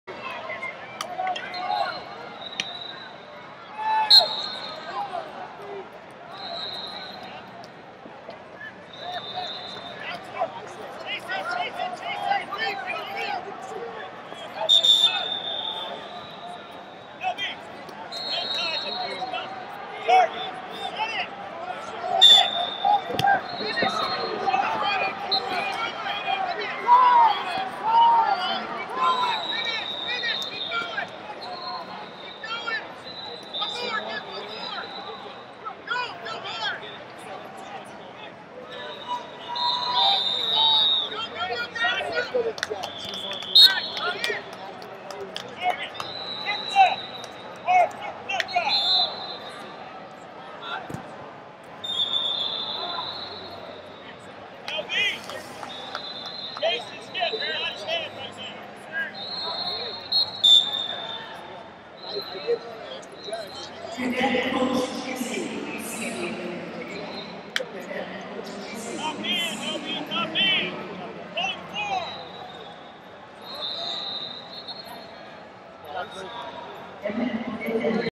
285 Lbs Rnd Of 32 Ethan DeRoche Montana Vs Jacob Barnes Oregon
2019 USMC USAW Junior And Cadet National Championships - 285 Lbs Rnd Of 32 Ethan DeRoche Montana Vs Jacob Barnes Oregon